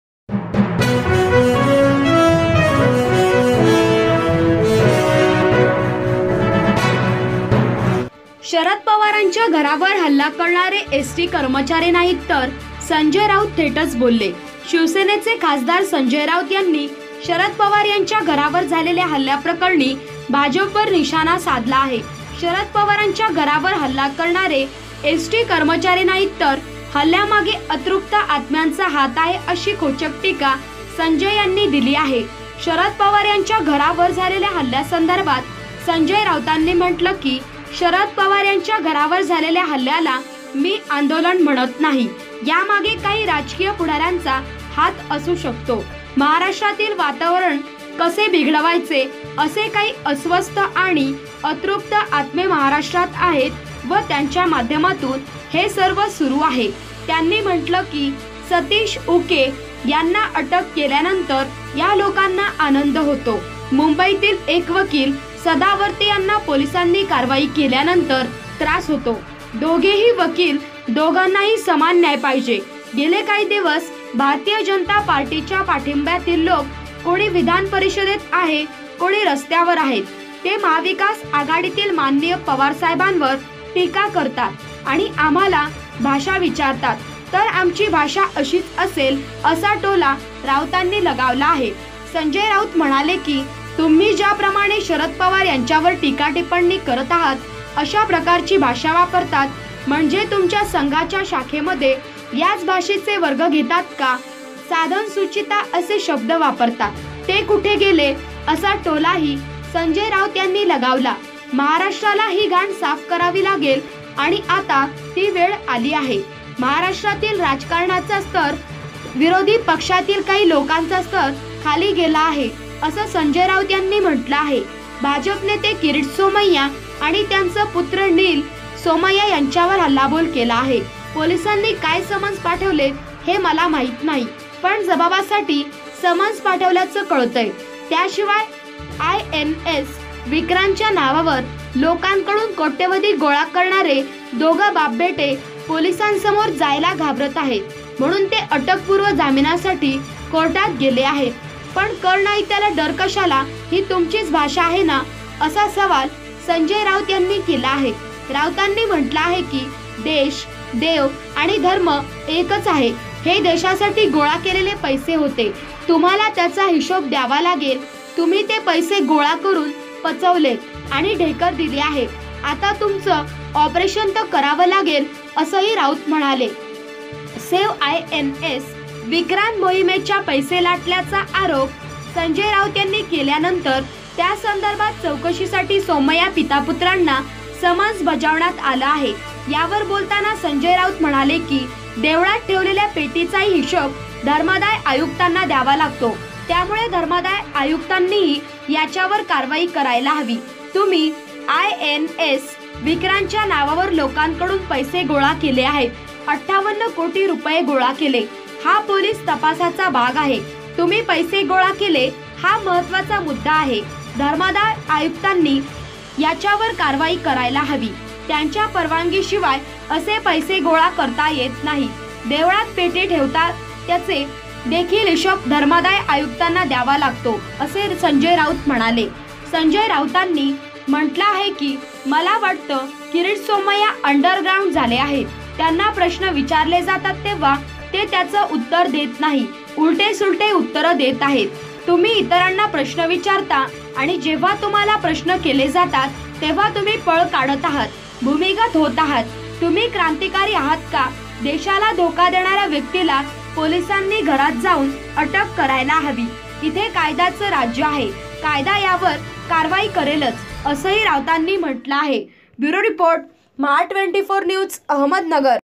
महा 24 News | शरद पवारांच्या घरावर हल्ला करणारे एसटी कर्मचारी नाहीत तर…, संजय राऊत थेटच बोलले
शरद पवारांच्या घरावर हल्ला करणारे एसटी कर्मचारी नाहीत तर…, संजय राऊत थेटच बोलले

  Shiv Sena Sanjay Raut On Sharad Pawar : शिवसेनेचे खासदार संजय राऊत यांनी शरद पवार यांच्या घरावर झालेल्या हल्लाप्रकरणी भाजपवर निशाणा साधला आहे. शरद पवारांच्या घरावर हल्ला करणारे एसटी कर्मचारी नाहीत तर हल्ल्यामागे अतृप्त आत्म्यांचा हात आहे, अशी खोचक टीका संजय यांनी दिली आहे. शरद पवार यांच्या घरावर झालेला हल्ल्यासंदर्भात संजय राऊतांनी म्हटलं की, शरद पवार यांच्या घरावर झालेला हल्ल्याला मी आंदोलन म्हणत नाही. यामागे काही राजकीय पुढाऱ्यांचा हात असू शकतो. महाराष्ट्रातील वातावरण बिघडवायचे आहे, असे काही असे अस्वस्थ आणि अतृप्त आत्मे महाराष्ट्रात आहेत व त्यांच्या माध्यमातून हे सर्व सुरू आहेत.

त्यांनी म्हटलं की, सतीश उके यांना अटक केल्यानंतर या लोकांना आनंद होतो. मुंबईतील एक वकील सदावर्ते यांना पोलिसांनी कारवाई केल्यानंतर त्रास होतो. दोघेही वकील, दोघांनाही समान न्याय पाहिजे. गेले काही दिवस भारतीय जनता पार्टीच्या पाठिंब्यातील लोक कोणी विधान परिषदेत आहेत, कोणी रस्त्यावर आहेत. ते महाविकास आघाडीतील माननीय पवार साहेबांवर टीका करतात आणि आम्हाला भाषा विचारतात. तर आमची भाषा अशीच असेल असा टोला राऊतांनी लगावला आहे.

संजय राऊत म्हणाले की, तुम्ही ज्या प्रमाणे शरद पवार यांच्यावर टीका टिप्पणी करत आहात. अशा प्रकारची भाषा वापरतात. म्हणजे तुमच्या संघाच्या शाखेमध्ये याच भाषेचे वर्ग घेतात का? साधनशुचिता असे शब्द वापरतात. ते कुठे गेले? असा टोलाही संजय राऊत यांनी लगावला. महाराष्ट्राला ही घाण साफ करावी लागेल आणि आता ती वेळ आली आहे. महाराष्ट्रातील राजकारणाचा स्तर नाही तर विरोधी पक्षातील काही लोकांचा स्तर खाली गेला आहे, असं संजय राऊत यांनी म्हटलं आहे.

भाजप नेते किरीट सोमय्या आणि त्यांचे पुत्र नील सोमय्या यांच्यावर हल्लाबोल केला आहे. पोलिसांनी काय समन्स पाठवलेत, हे मला माहिती नाही. पण जबाबासाठी समन्स पाठवल्याचं कळतंय. त्याशिवाय, आयएनएस विक्रांतच्या नावावर लोकांकडून कोट्यवधी गोळा करणारे दोघं बापबेटे पोलिसांसमोर जायला घाबरत आहेत. म्हणून ते अटकपूर्व जामिनासाठी कोर्टात गेले आहेत. पण कर नाही तर डर कशाला ही तुमचीच भाषा आहे ना? असा सवाल राऊत यांनी केला आहे. राऊतांनी म्हटलं की, देश, देव आणि धर्म एकच आहे. हे देशासाठी गोळा केलेले पैसे होते. तुम्हाला त्याचा हिशोब द्यावा लागेल. तुम्ही ते पैसे गोळा करून पचवलेत आणि ढेकर दिली आहे. आता तुमचं ऑपरेशन तर करावं लागेल, असंही राऊत म्हणाले.

सेव्ह INS विक्रांत मोहिमेच्या पैसे लाटल्याचा आरोप संजय राऊत यांनी केल्यानंतर त्यासंदर्भात चौकशीसाठी सोमय्या पितापुत्रांना समन्स बजावण्यात आलं आहे. यावर बोलताना संजय राऊत म्हणाले की, देवळात ठेवलेल्या पेटीचाही हिशोब धर्मादाय आयुक्तांना द्यावा लागतो. त्यामुळे धर्मादाय आयुक्तांनीही यांच्यावर कारवाई करायला हवी. तुम्ही आयएनएस विक्रांतच्या नावावर लोकांकडून पैसे गोळा केले आहेत. 58 कोटी रुपये गोळा केले. हा पोलीस तपासाचा भाग आहे. तुम्ही पैसे गोळा केले, हा महत्त्वाचा मुद्दा आहे. धर्मादाय आयुक्तांनी देखील यांच्यावर कारवाई करायला हवी. त्यांच्या परवानगीशिवाय असे पैसे गोळा करता येत नाहीत. देवळात पेटी ठेवता त्याचा देखील हिशोब धर्मादाय आयुक्तांना द्यावा लागतो, असं राऊत म्हणाले.

संजय राऊतांनी म्हटलं की, मला वाटतं किरीट सोमय्या अंडरग्राउंड झाले आहेत. त्यांना प्रश्न विचारले जातात तेव्हा ते त्याचं उत्तर देत नाहीत. उलटे-सुलटे उत्तरं देत आहेत. तुम्ही इतरांना प्रश्न विचारता आणि जेव्हा तुम्हाला प्रश्न केले जातात, तेव्हा तुम्ही पळ काढत आहात. भूमिगत होत आहात. तुम्ही क्रांतिकारी आहात का? देशाला धोका देणाऱ्या व्यक्तीला पोलिसांनी घरात जाऊन अटक करायला हवी. इथे कायद्याचं राज्य आहे. कायदा यावर कारवाई करेलच, असंही राऊतांनी म्हटलं आहे.

• Ahmednagar News

• Corona Update

• Ahmednagar Corona

Top Video

Live Stream